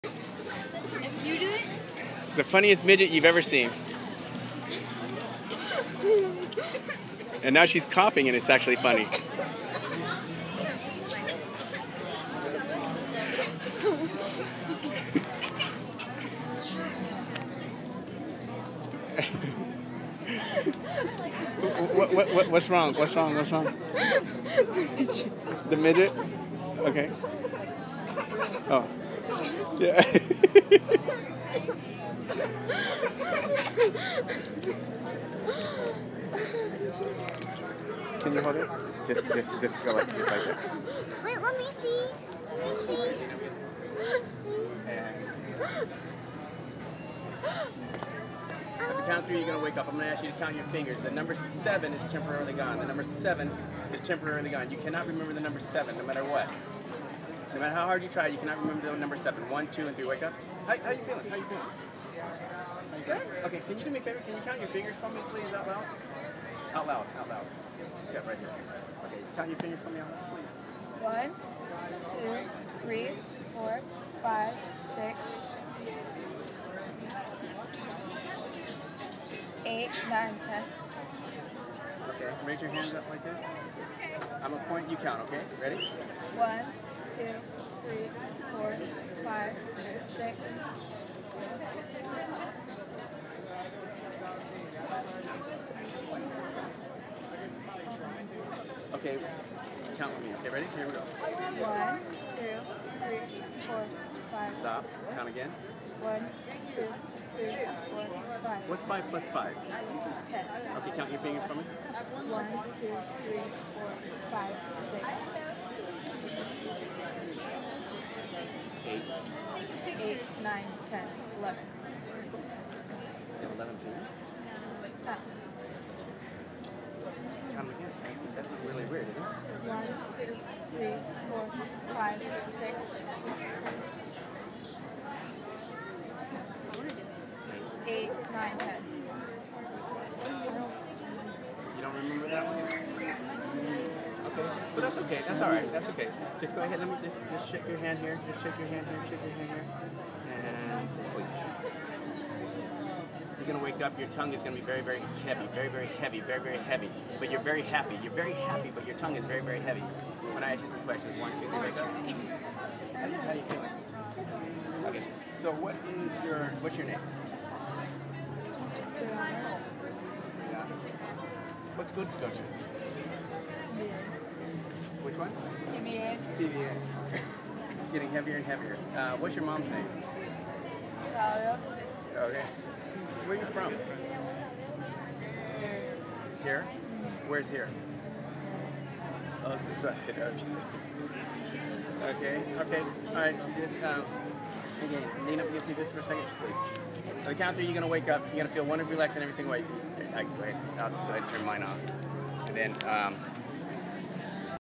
Britney having some fun
Britney wanted to be hypnotized and I made her wish, she's a great subject and a lovely young lady to work with.  Good luck Britney in your quest and may all of your dreams come true.